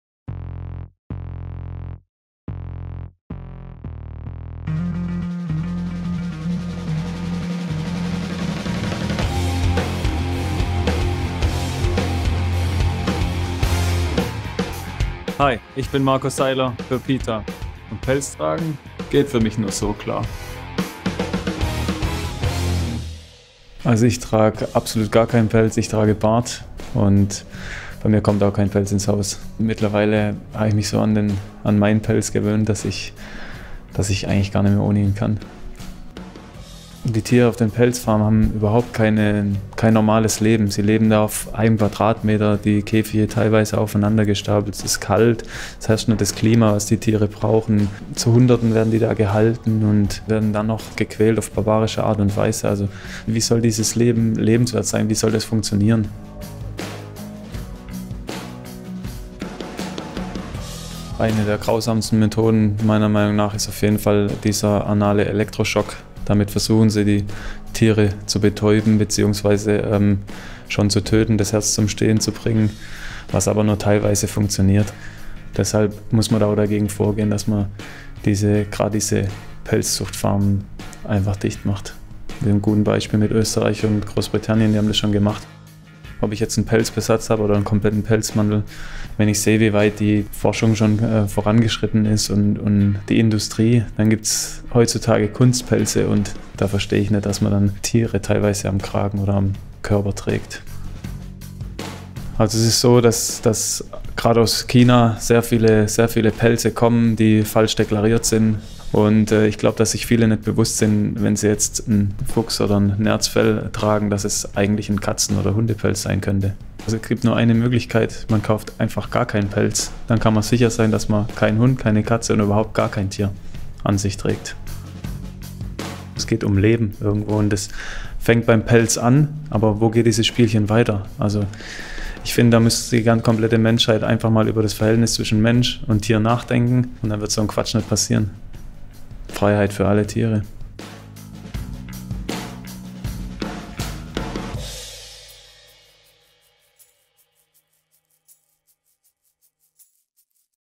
Marco Sailer - So trägt Man(n) Pelz! / PETA
Marco Sailer fällt auf – der Stürmer des SV Darmstadt hat nicht nur den üppigsten Vollbart der Bundesliga, sondern auch ein großes Herz für Tiere. Seit knapp einem Jahr lebt der 30-Jährige vegan. Nun zeigt sich Sailer in der neuen Kampagne für  PETA mit dem Claim: „So trägt Mann Pelz“. Im dazugehörigen Making-of-Video spricht der Profifußballer über die untragbaren Zustände auf Pelzfarmen und erklärt, warum Österreich und Großbritannien Deutschland einiges voraushaben.